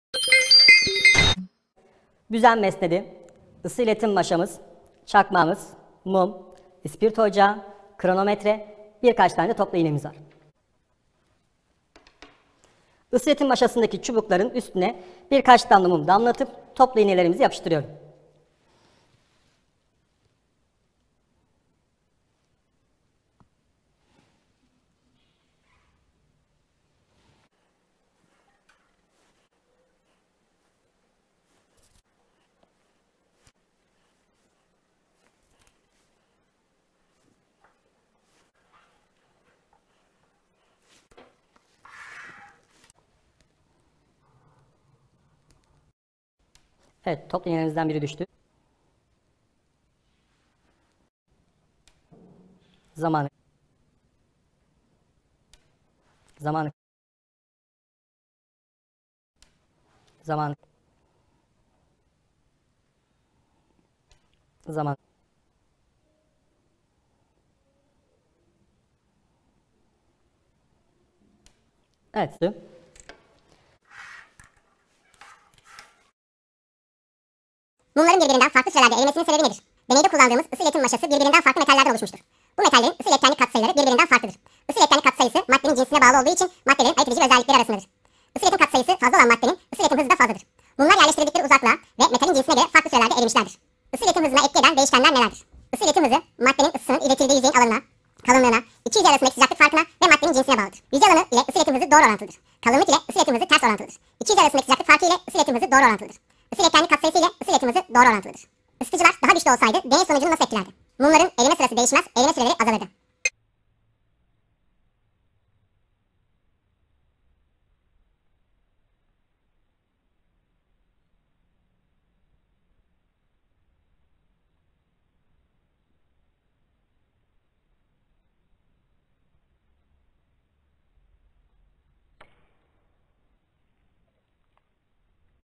Deney: Katılarda Isı İletim Hızı ve Bağlı Değişkenler (kaynağa uzaklık, katının cinsi, ısıtıcı gücü)
9.Sınıf / TYT / YKS